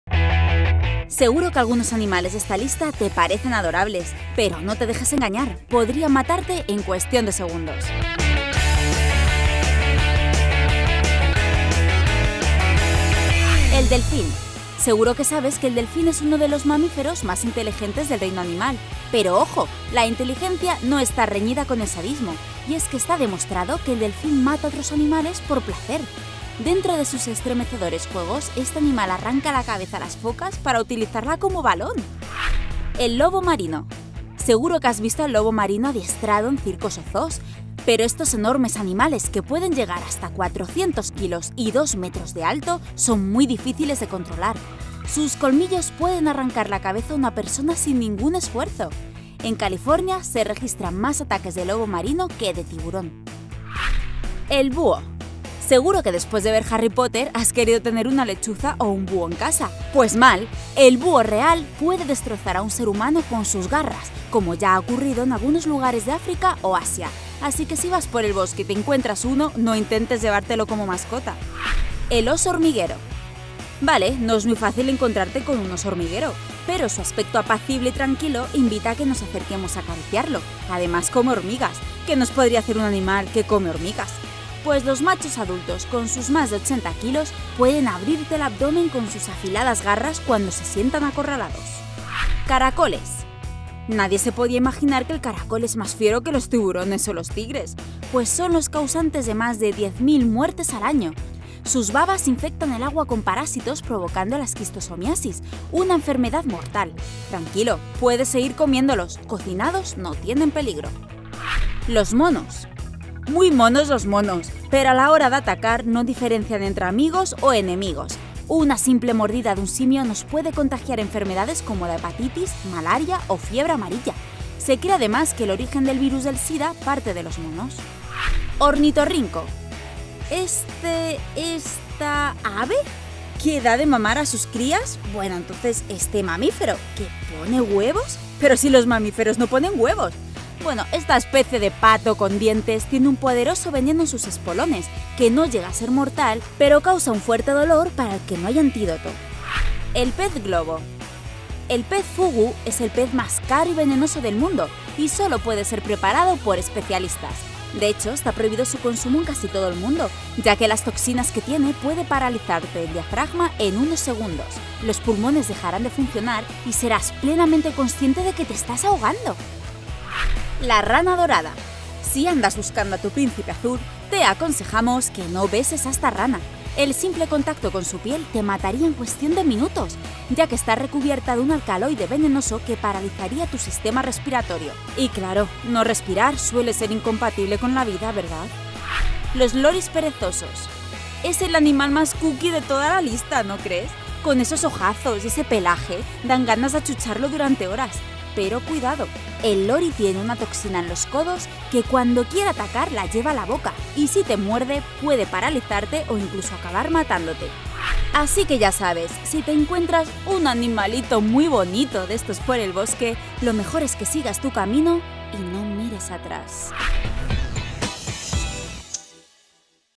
Los 10 animales que no deberias tener en casa ! vea porque se lo decimos..
Son tan monos y adorables! Estos 10 bonitos animales son más peligrosos de lo que parecen, incluso podrían matarte...